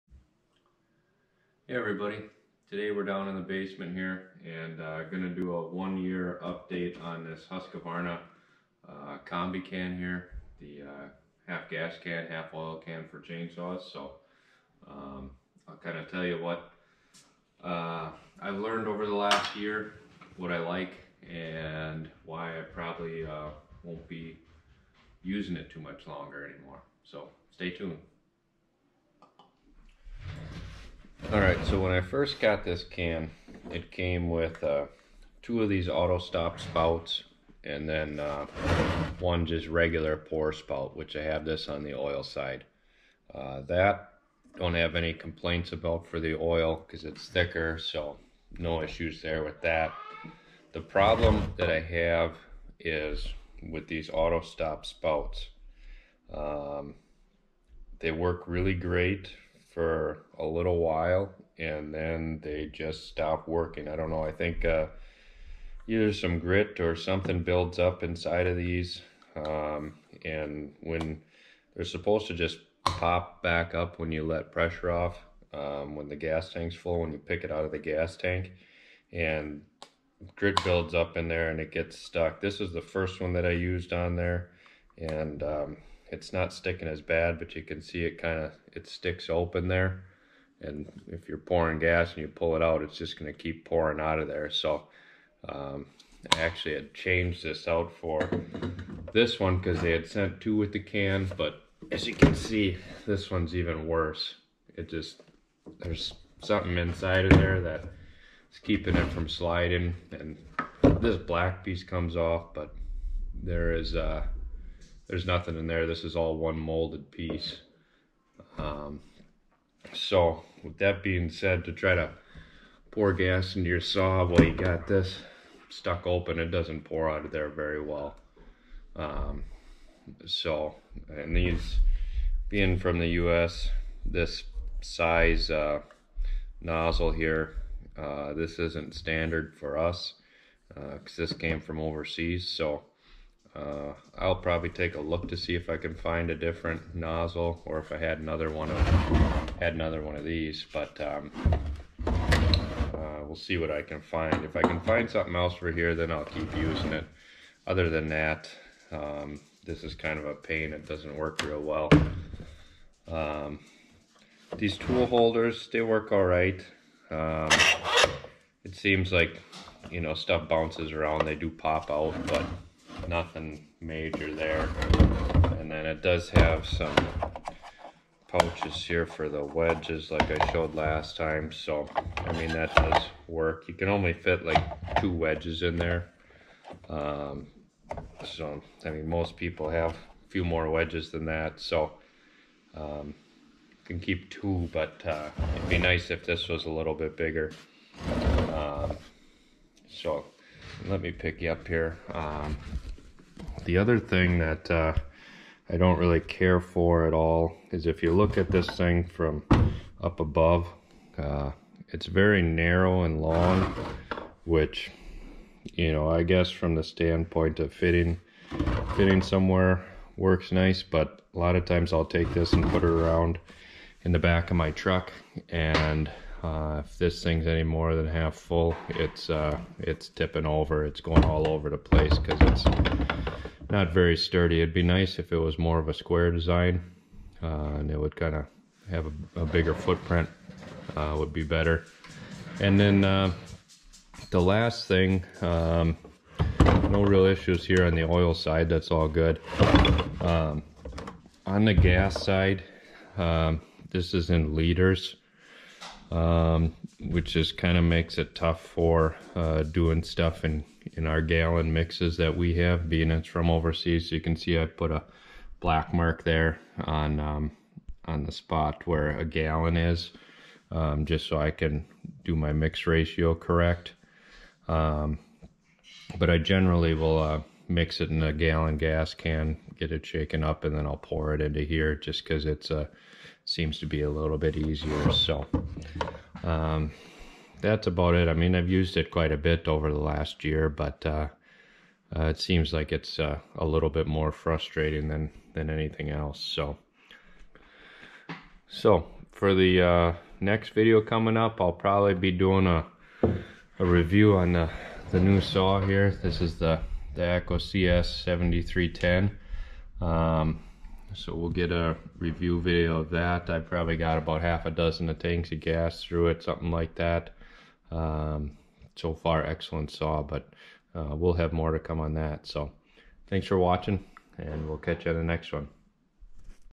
014 - Husqvarna Combi Can Update, Echo CS-7310
1 year review of the Husqvarna gas and bar oil can